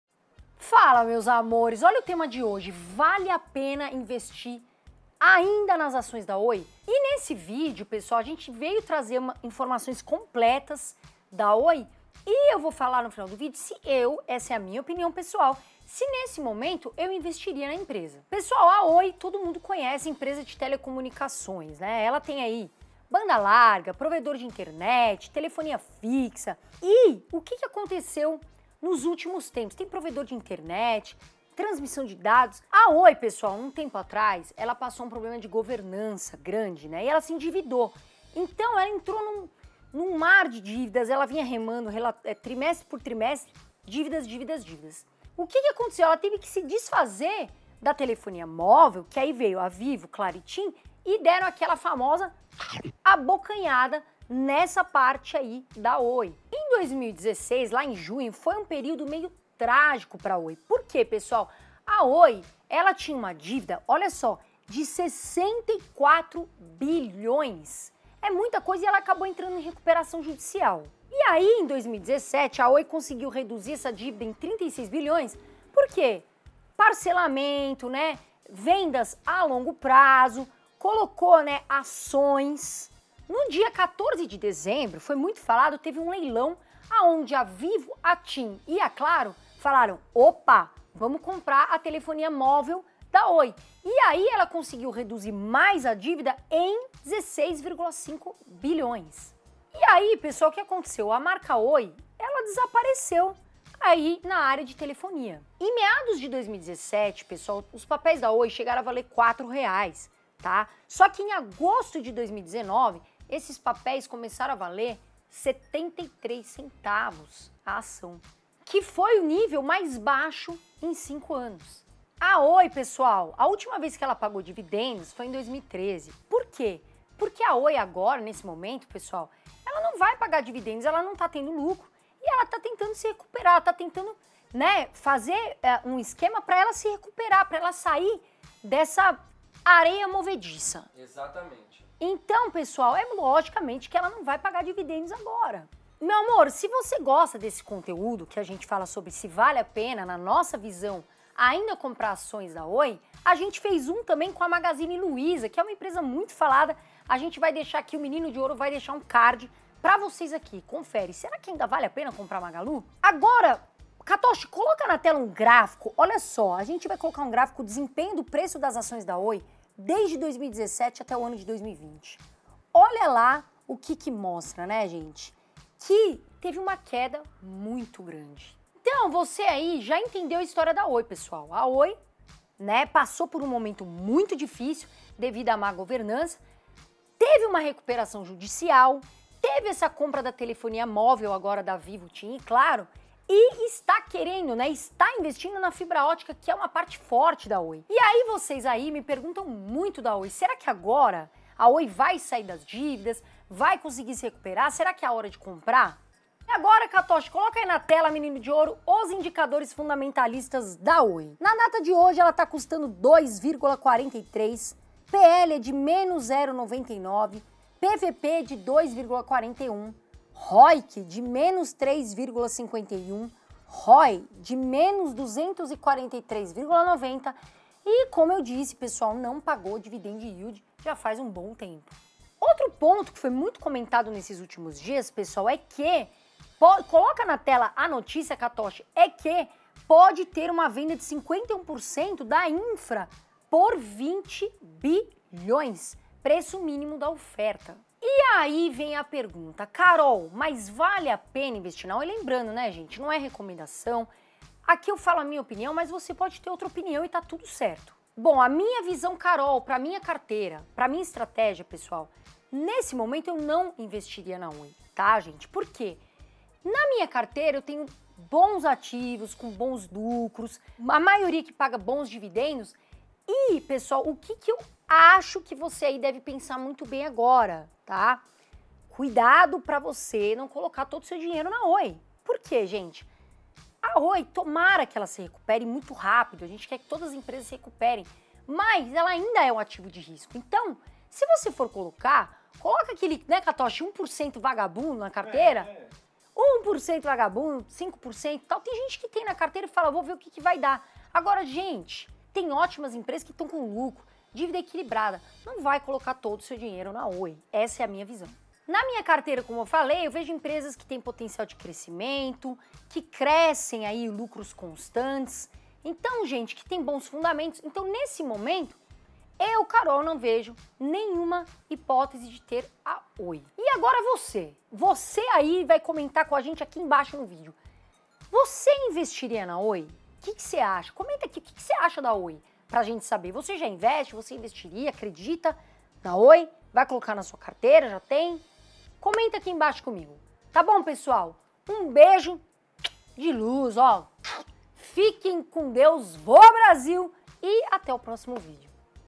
OIBR3 & OIBR4: Vale a pena COMPRAR as ações da OI?
A Oi é uma empresa que tem sido bastante falada nos últimos tempos, são investidores falando bem, outros falando mal, alguns preocupados com as suas posições, outros querendo tirar um lucro caso a empresa venha se recuperar. Será que vale a pena investir nas ações da empresa?

A Oi é uma empresa brasileira de telecomunicações. Ela oferece serviços de telefonia fixa, banda larga, TV por assinatura, transmissão de dados e provedor de internet. Também oferece serviços corporativos e de atacado. Adicionalmente, a Oi possui 2 milhões de pontos de Wi-Fi.

A Oi entrou em recuperação judicial em junho de 2016, após acumular dívida bruta de R$ 64 bilhões com cerca de 55 mil credores. A aprovação do plano de recuperação ocorreu em dezembro de 2017 e reduziu essa dívida em R$ 36 bilhões por meio de descontos, parcelamentos no longo prazo e conversão de créditos em ações.

🚀Quer receber análises de ações, ETFs e ações no exterior, fundos imobiliários? Entre na nossa comunidade VIP, totalmente gratuito:

NA SUA OPINIÃO VALE A PENA INVESTIR NA OI? 👇🏼🇧🇷